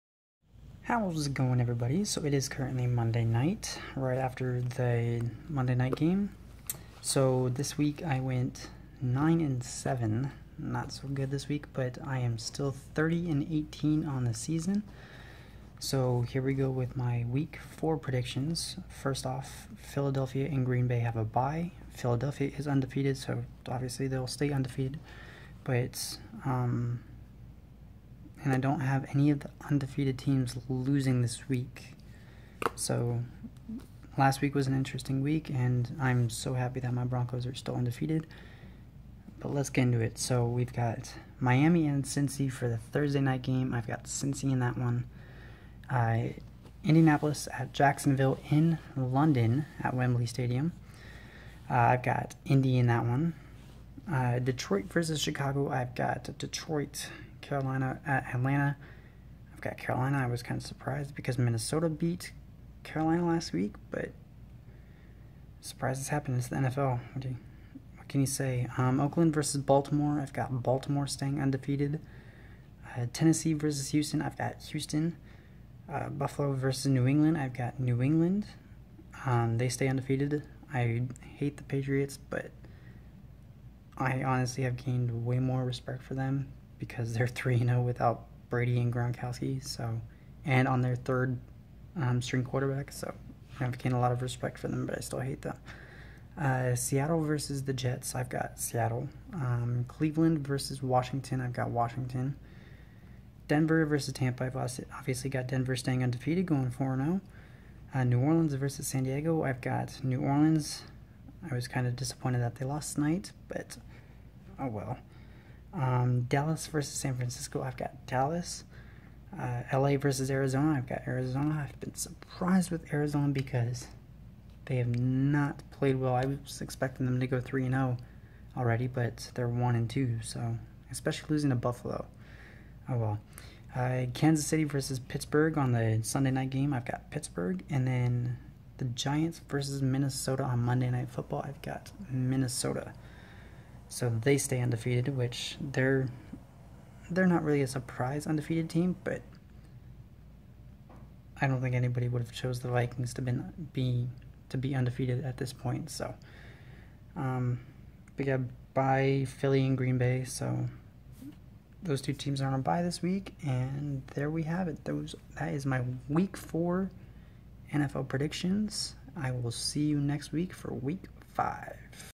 NFL Week 4 Predictions
Last week: 9-7
Overall: 30-18
Hoping for a bounce back week after losing 7 games in Week 3.